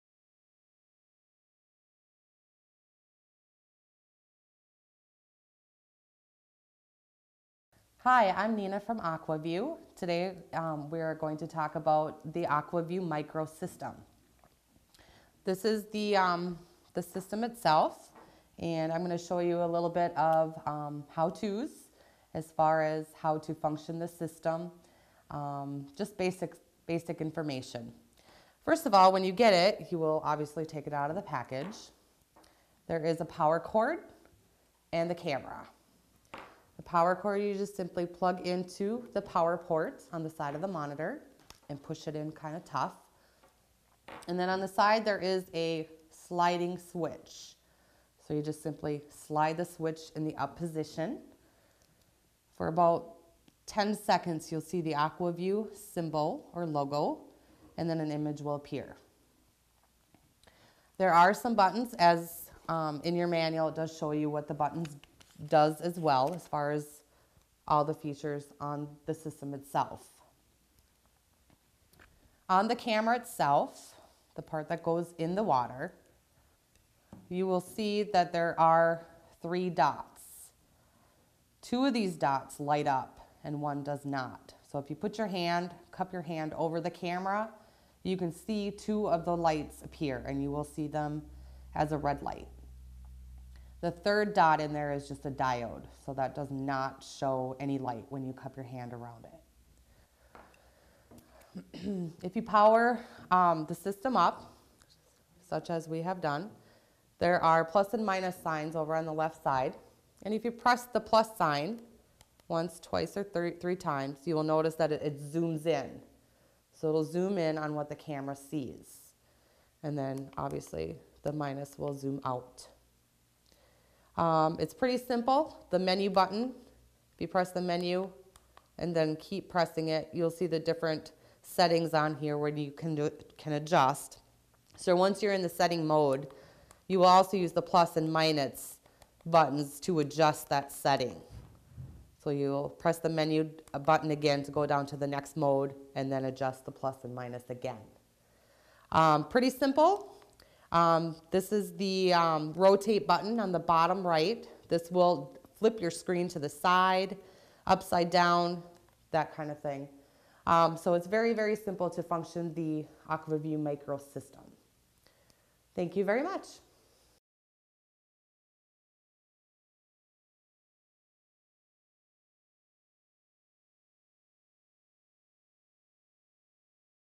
Original Aqua-Vu - Micro Basic Set Up
Nina from Aqua-Vu demonstrates basic set-up and system functions of the Original AV Micro Underwater camera system.